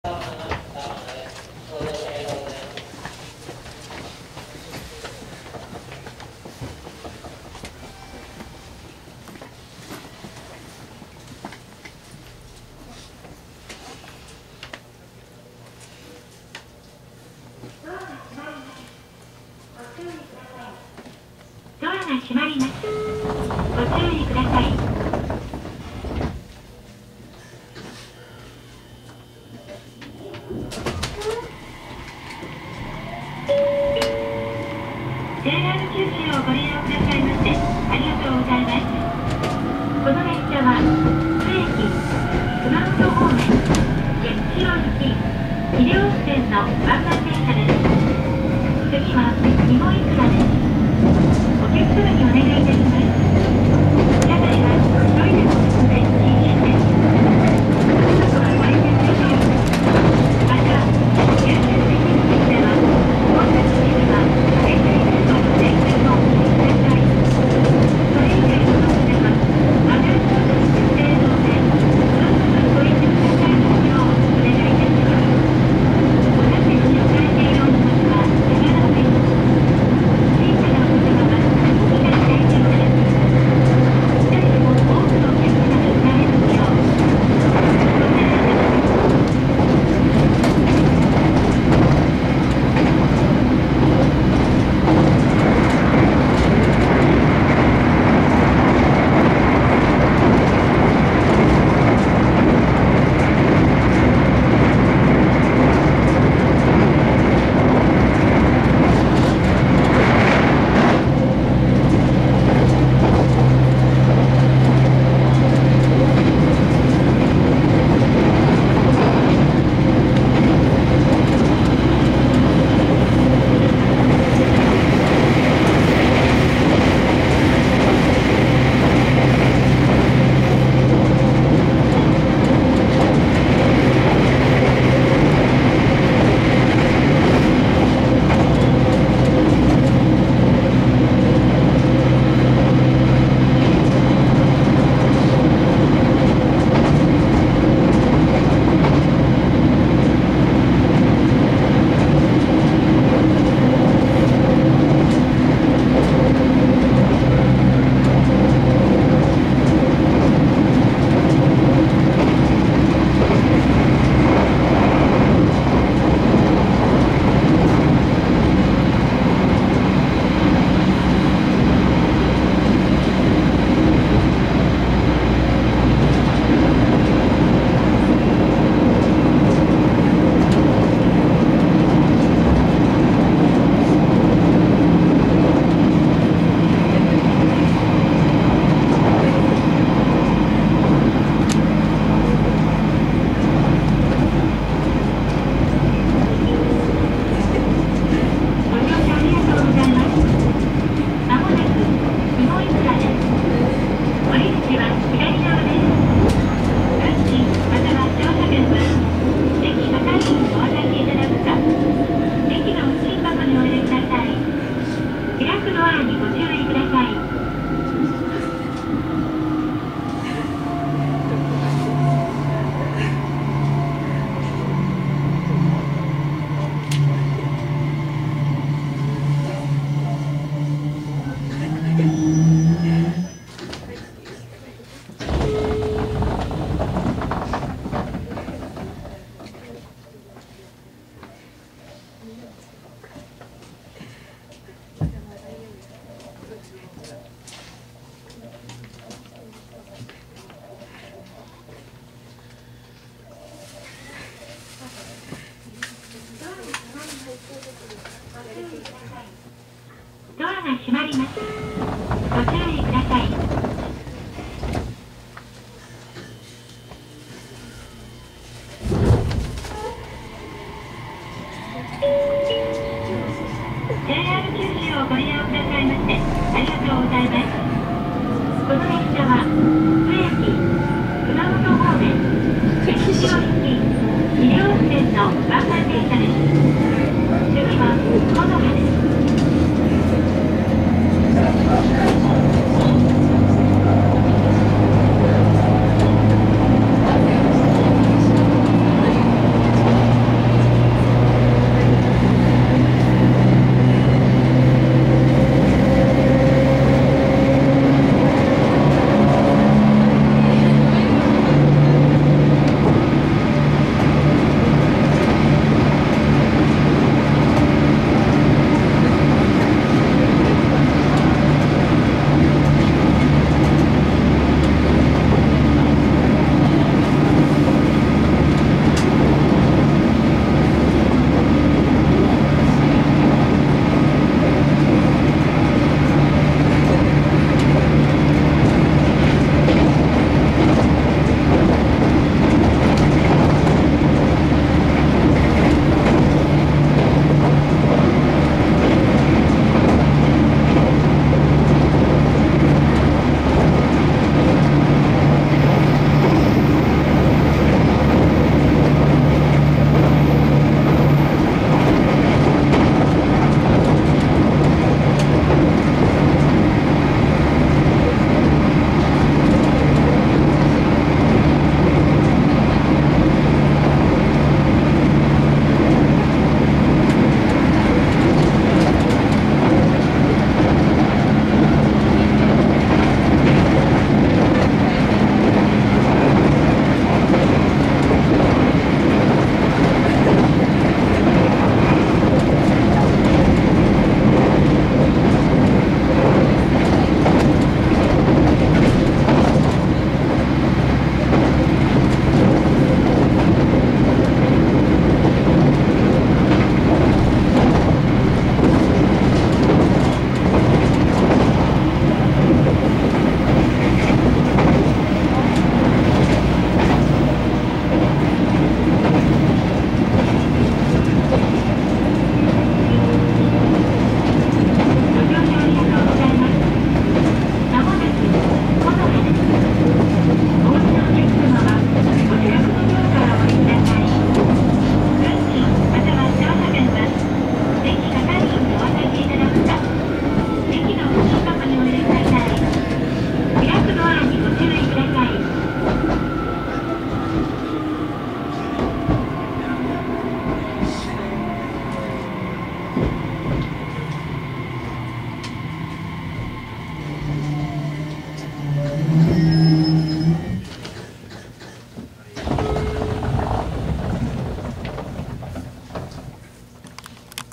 JR鹿児島本線 車窓 玉名駅～木葉駅（815系普通ワンマン)
2020年2月23日　JR鹿児島本線 車窓  玉名駅～木葉駅 815系普通ワンマン（NT014編成普通ワンマン八代2両）